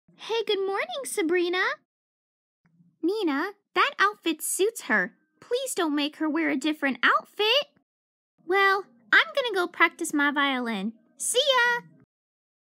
Lines for Leafy Yesh (REUPLOAD)
For Leafy Yesh’s upcoming Gacha Club series Element Diamond Girls

I’m the VA for Scarlet.

(Had to keep reuploading this cuz of tech problems)